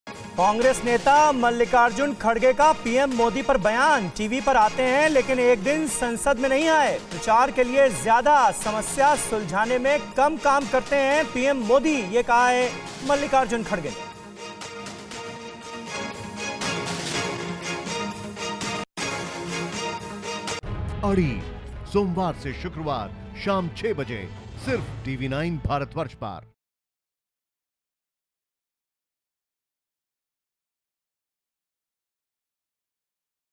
Congress नेता Mallikarjun Kharge का PM Modi पर बयान, कहा- टीवी पर आते हैं लेकिन संसद में नहीं आए
कांग्रेस नेता मल्लिकार्जुन खड़गे ने  पीएम मोदी पर बयान देते हुए कहा कि पीएम मोदी टीवी पर तो आते हैं. लेकिन एक दिन संसद में नहीं आए. प्रचार  के लिए ज्यादा समस्या पर कम काम करते हैं.